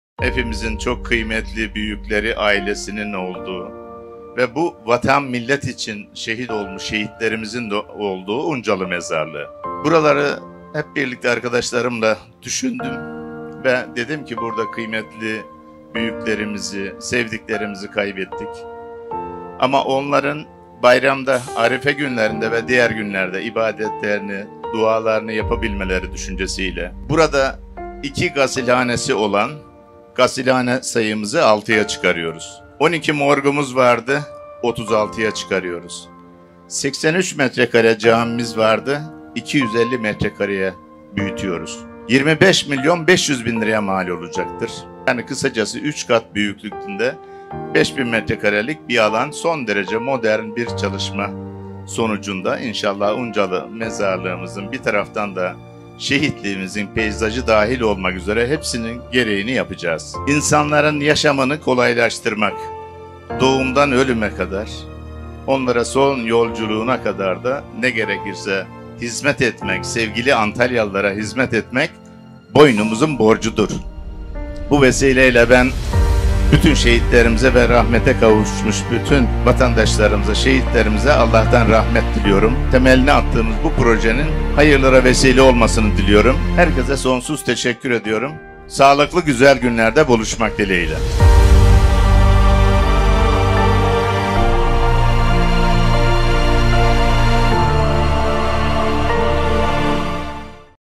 Uncalı Mezarlığı Projesi’nin Temeli Atıldı
Uncalı Mezarlığı daha modern bir hale geliyor.

Başkan Muhittin Böcek Uncalı Mezarlığı Projesi’nin temel atma törenini gerçekleştirdi.

Oynatma Listeleri;